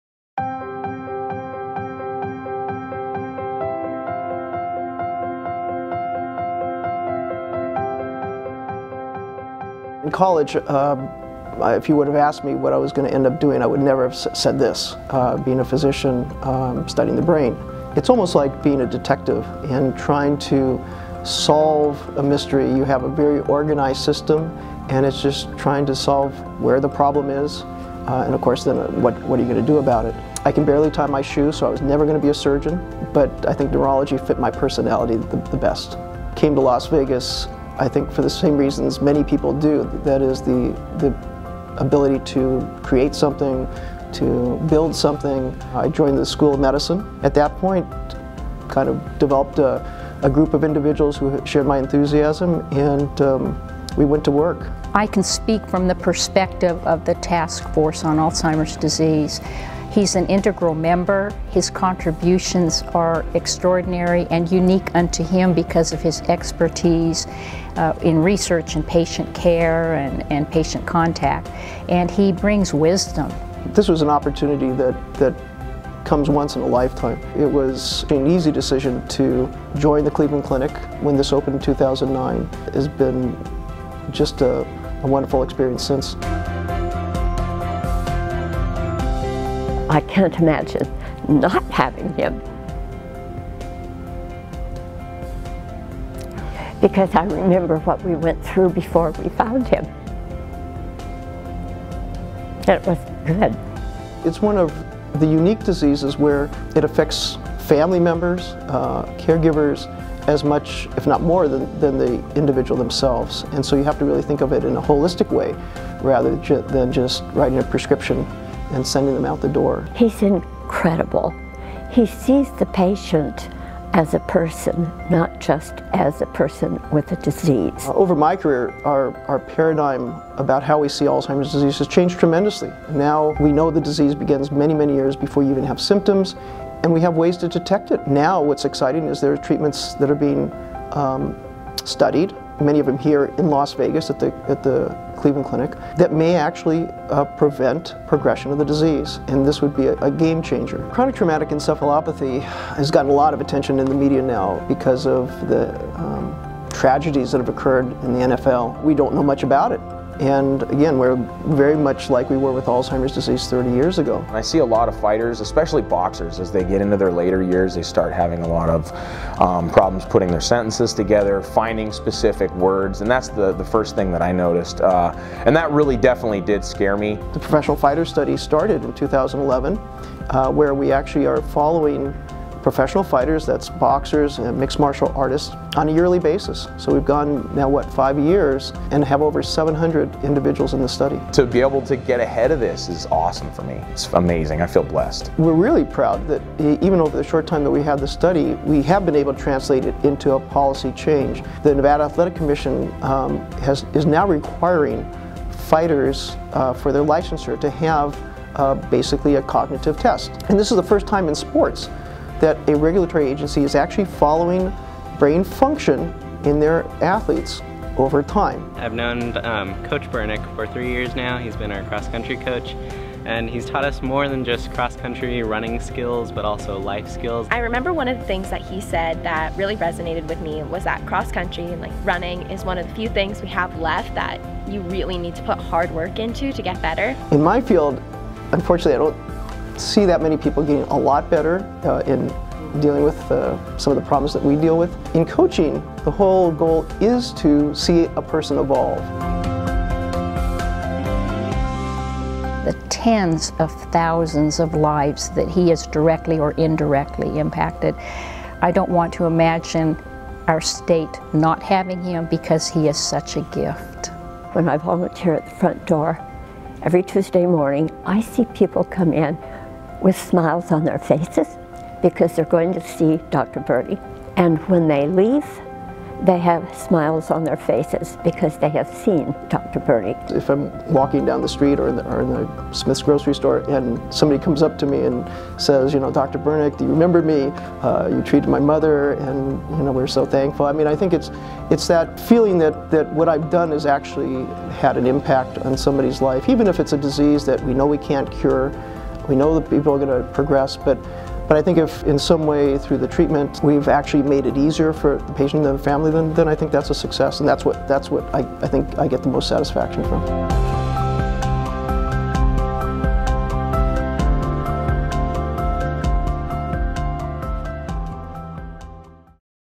Charles Bernick, MD, MPH | 2016 Inspired Excellence in Healthcare Award Recipient
In celebration of the 5th Annual Dr. Roy W. Martin Inspired Excellence in Healthcare Awards, we sit down with award recipient Dr. Charles Bernick.

Dr. Charles Bernick has been a pioneer in the research and treatment of Alzheimer’s disease in Nevada and nationally. As a physician with the Cleveland Clinic Lou Ruvo Center for Brain Health, Dr. Bernick has also spearheaded the study of CTE, Chronic Traumatic Encephalopathy, as well as the long-term impact of concussions and repetitive head trauma in contact sports.

Thank you, Dr. Charles Bernick!